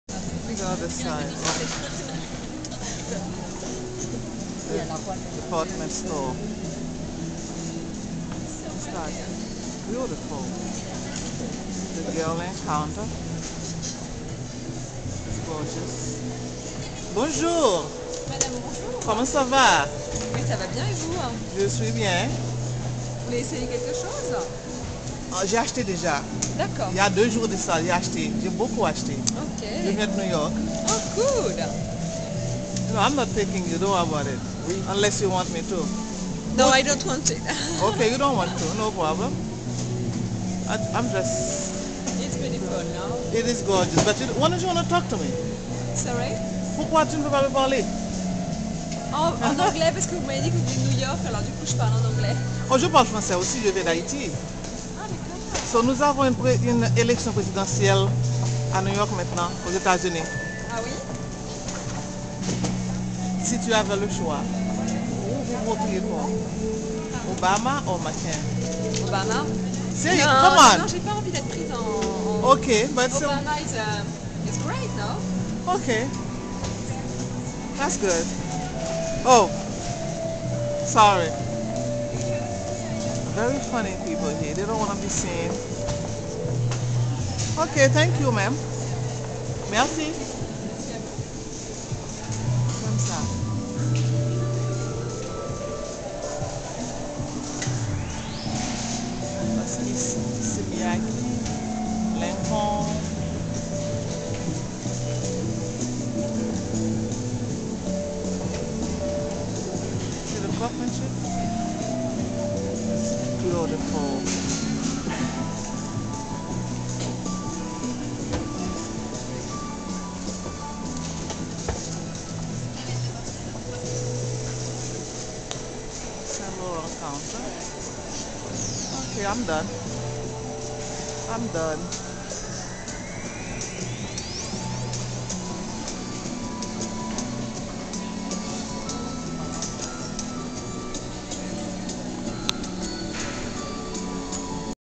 Le Printemps
The # 1 perfume store in the world, and it is located in the heart of Paris just a block away from Gallery Lafayette.